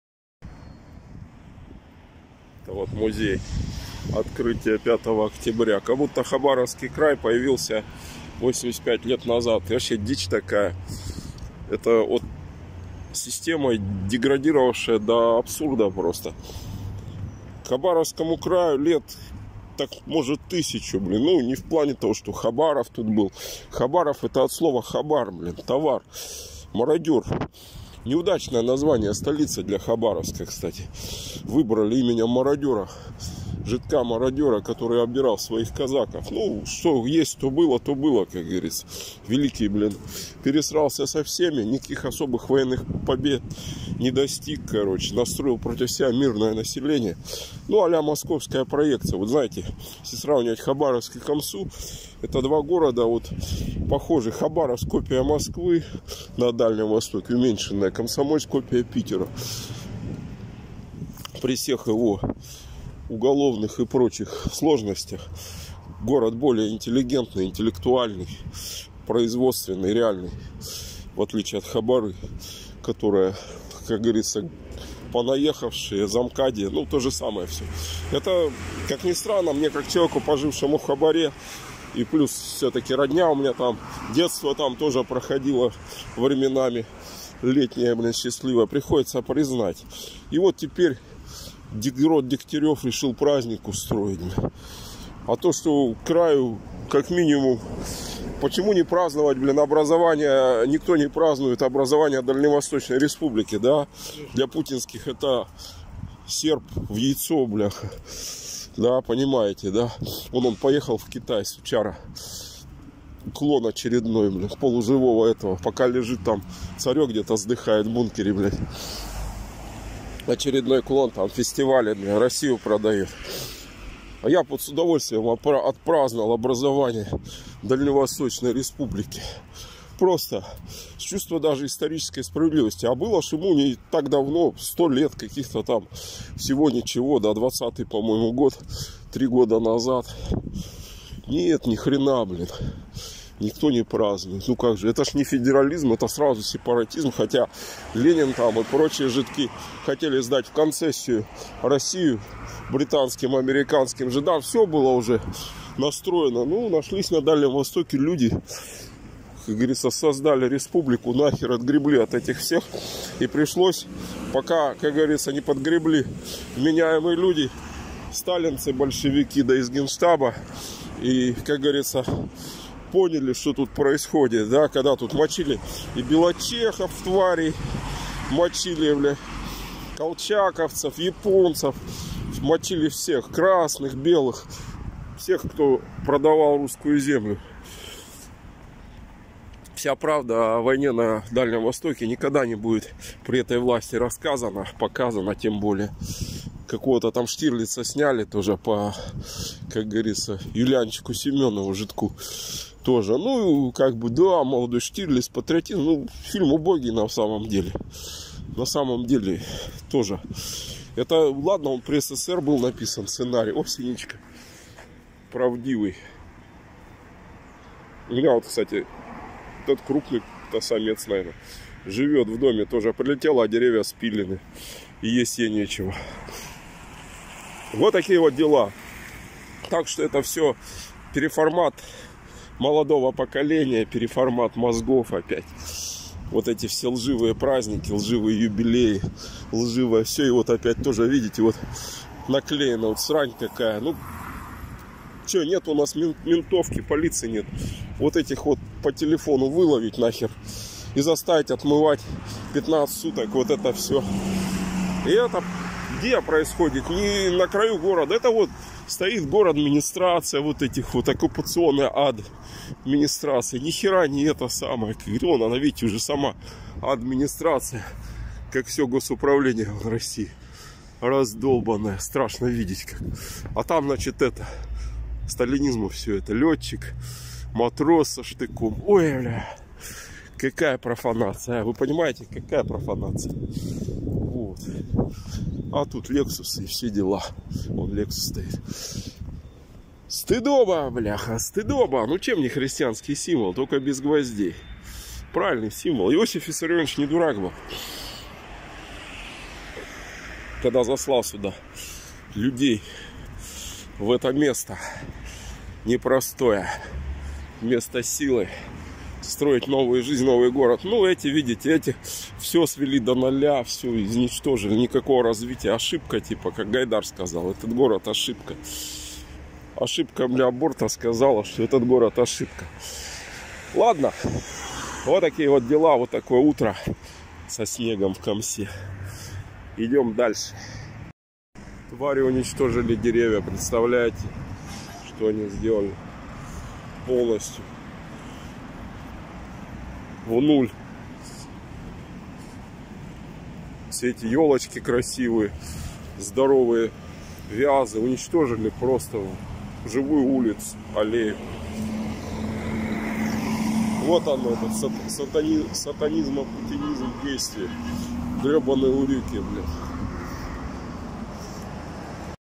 Комсомольск напротив райкома партии
стеб без комментов но...крепким боцманским словом в финале...